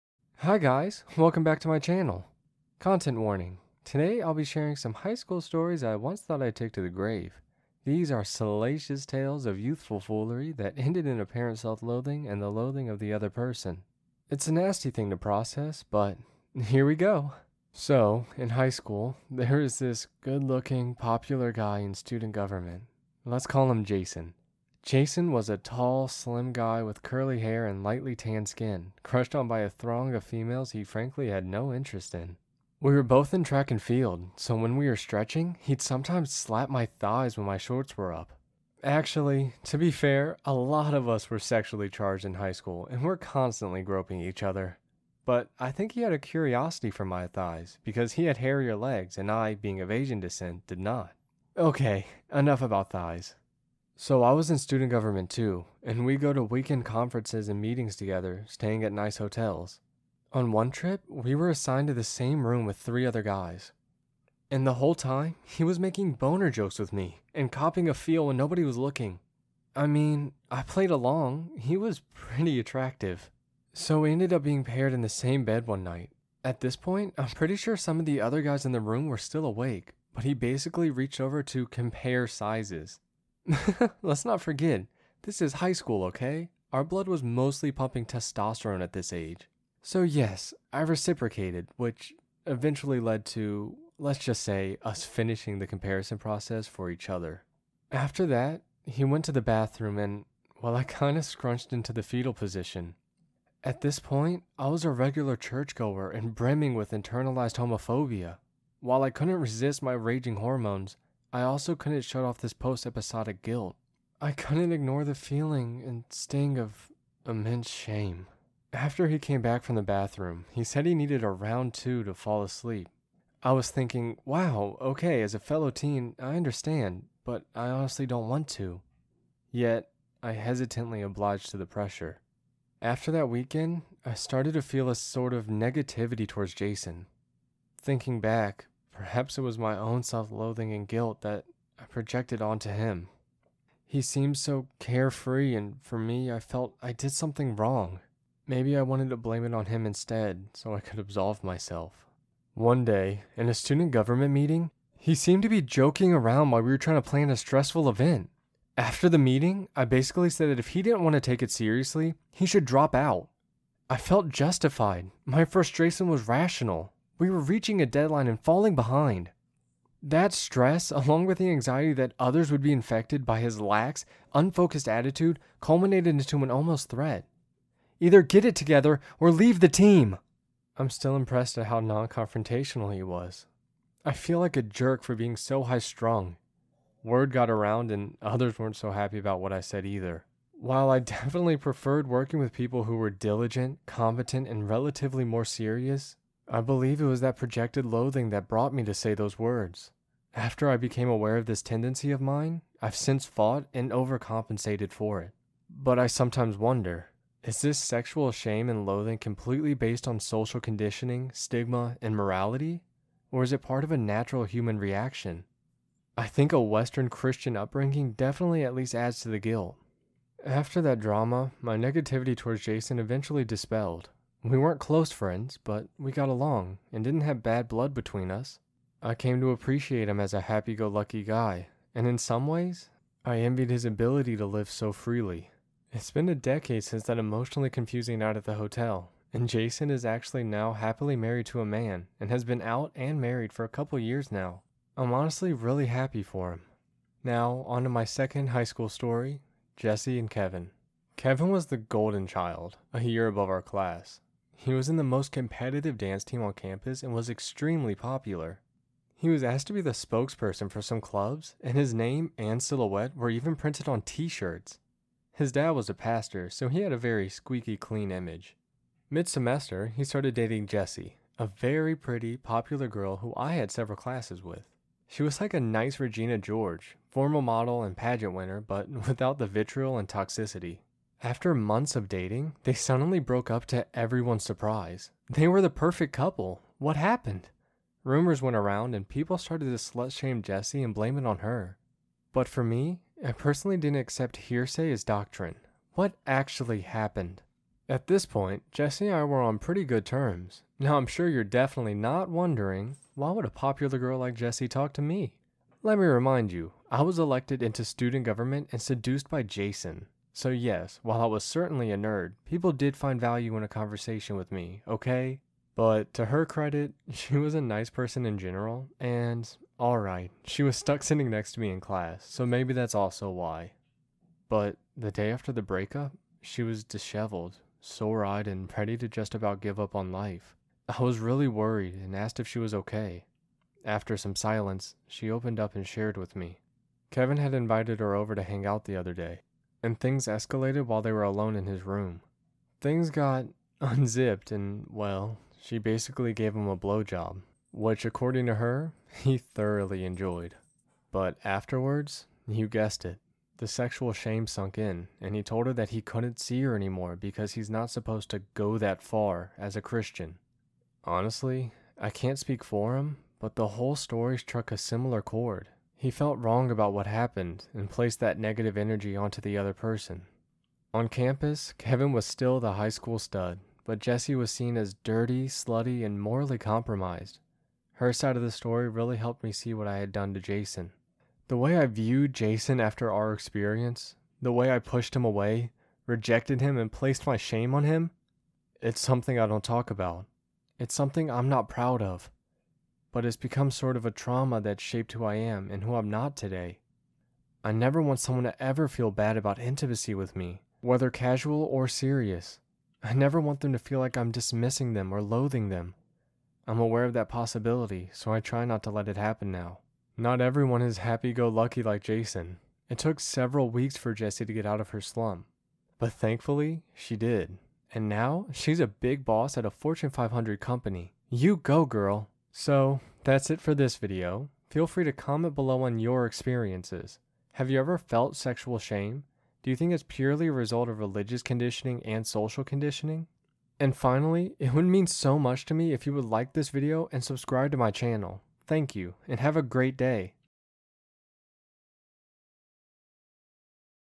Guilt After Sexual Encounters In High School (Animated) | Postcoital Self-Loathing, Gay Christian
Have you ever felt guilt, shame, or self-loathing after doing something sexual with someone else? Have you ever directed those feelings negatively toward to the other person?

In this video, I share two stories from my high school experience where something sexual ended up with one person projecting dirtiness and loathing onto the other person.

The first story is one of my first gay experiences in high school. The second was involved two very popular classmates. But one thing was in common: the ones feeling the guilt and shame (aka me) were Christian. Perhaps it was my Christian upbringing that made me feeling immense guilt, and it seemed like the other person was the source of this guilt.

I ended up lashing back at him, and to be honest, I feel really bad now for doing that. What were you experiences like?

Voiced by HyugaVA
Animated by Vidrienna

postcoital tristesse, postcoital dysphoria, anxiety after sex, depression after sex, high school sexual encounters, lgbtq stories, gay animation